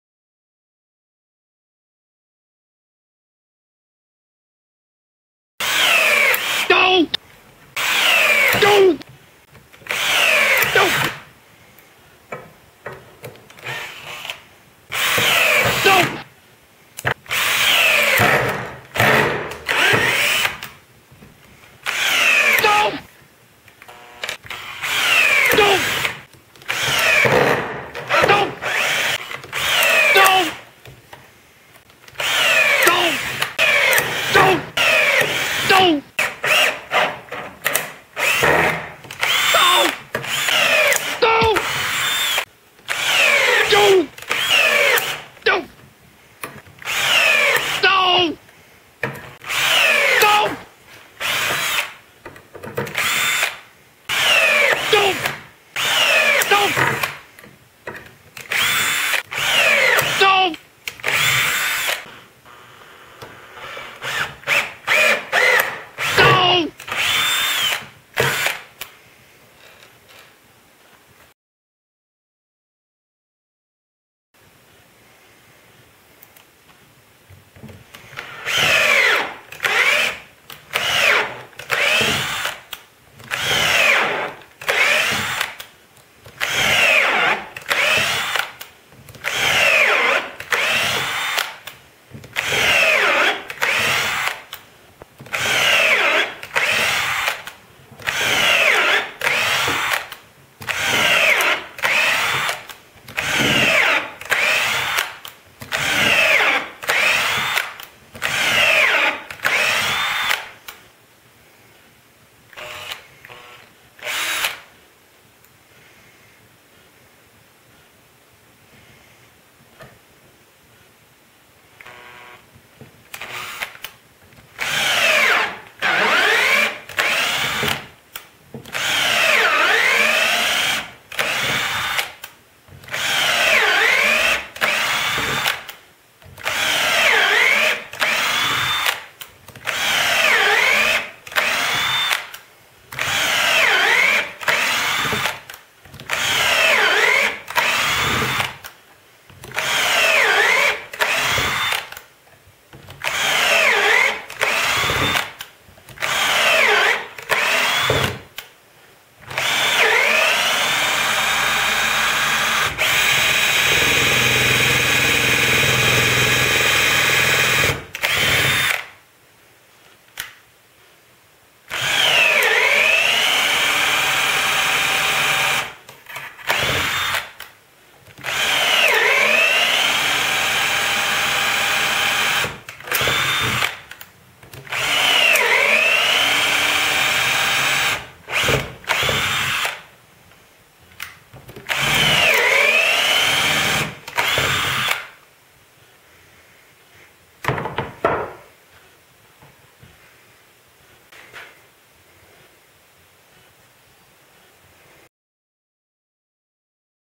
Drywall Screws VS Wood Screws
An unscientific, but effective demonstration of why drywall screws are not very well suited to woodworking. The wood used is hard maple.